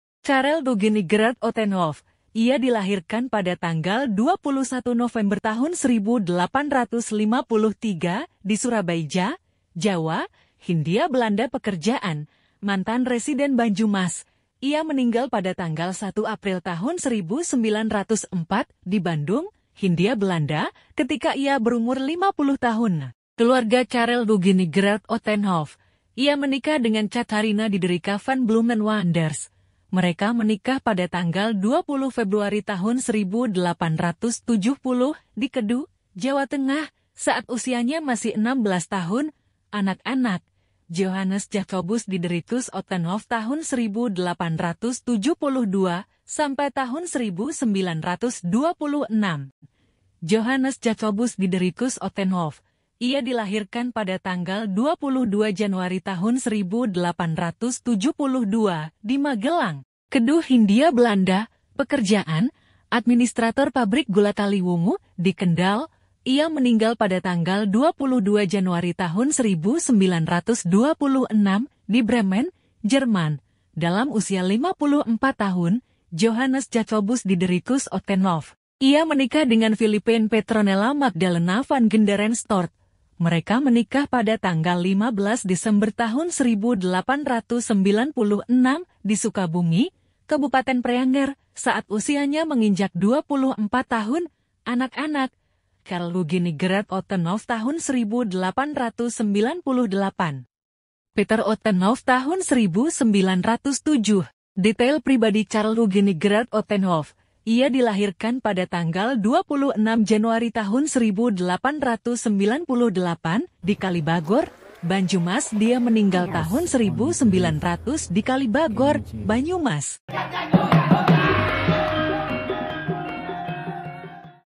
Famili Ottenhoff di Banjoemas
Carel Eugene Gerard Ottenhoff

Ia dilahirkan pada tanggal 21 November 1853 di Soerabaija, Jawa, Hindia Belanda.
Pekerjaan : mantan residen Banjoemas.
Ia meninggal pada tanggal 1 April 1904 di Bandung, Hindia Belanda, ketika ia berumur 50 tahun.

Keluarga Carel Eugene Gerard Ottenhoff
Ia menikah dengan Catharina Diederika van Bloemen Waanders.

Mereka menikah pada tanggal 20 Februari 1870 di Kedoe, Jawa Tengah, saat usianya masih 16 tahun.

Anak-anak :

Johannes Jacobus Diedericus Ottenhoff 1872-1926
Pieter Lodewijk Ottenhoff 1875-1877
Karel Ottenhoff 1878-?
Catharina Ottenhoff 1880-?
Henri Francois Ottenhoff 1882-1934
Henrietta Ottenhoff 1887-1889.

Johannes Jacobus Diedericus Ottenhoff
Ia dilahirkan pada tanggal 22 Januari 1872 di Magelang, Kedoe Hindia Belanda.

Pekerjaan: Administrator pabrik gula Kali Woengoe di Kendal.
Ia meninggal pada tanggal 22 Januari 1926 di Bremen, Jerman, dalam usia 54 tahun.

Ia menikah dengan Philippine Petronella Magdalena van Genderen Stort.

Mereka menikah pada tanggal 15 Desember 1896 di Soekaboemi, Kabupaten Preanger, saat usianya menginjak 24 tahun.

Anak-anak :
Carl Eugene Gerard Ottenhoff 1898-?
Peter Ottenhoff 1907-

Detail pribadi Carl Eugene Gerard Ottenhoff
Ia dilahirkan pada tanggal 26 Januari 1898 di Kalibagor, Banjoemas.
Dia meninggal di Kalibagor, Banyumas.

Peter Ottenhoff
Ia lahir pada tanggal 17 Februari 1907 di Kendal, Semarang, Jawa, Hundia Belanda.
Jam lima sore

Anak dari Johannes Jacobus Diedericus Ottenhoff dan Philippine Petronella Magdalena van Genderen Stort.

Catatan mengenai Peter Ottenhoff

Gulungan film 1357803, 1907, akte 25
Dilahirkan 17 Februari 1907 di rumah dokter Johan Wilhelm Adolph Schmidt, di Semarang (deklarasi 21 Februari 1907); Orangtua: Johan Jacob Diedericus Ottenhof, 65 tahun, kepala administrator pabrik gula Kalimoengoen di Kendal dan istrinya Philippine Petronella Magdalena van Genderen Stort
Keduanya tinggal di Kendal.